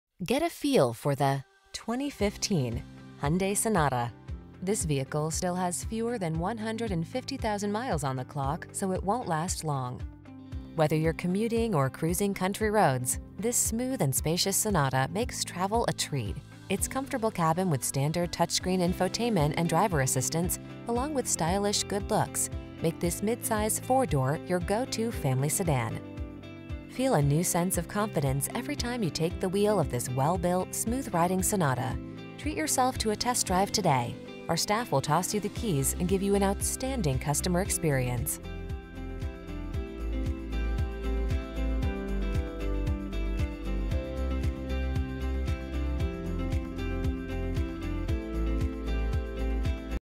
2015 Hyundai Sonata Miami Shores, Aventura, Miami Beach, Miami Lakes, Miami, FL 262379CF
Shale Gray Metallic Used 2015 Hyundai Sonata available in Miami Shores, Florida at Tropical Chevrolet. Servicing the Aventura, Miami Beach, Miami Lakes, Miami, FL area.

Tropical Chevrolet
8880 Biscayne Blvd

Shale Gray Metallic 2015 Hyundai Sonata SE FWD 6-Speed Automatic with Shiftronic 2.4L I4 DGI DOHC 16V ULEV II 185hpbrbr25/37 City/Highway MPGbrbrAwards:br  * 2015 KBB.com Best Buy Awards Finalist   * 2015 KBB.com 10 Most Comfortable Cars Under $30,000   * 2015 KBB.com 15 Best Family Cars   * 2015 KBB.com 10 Best Sedans Under $25,000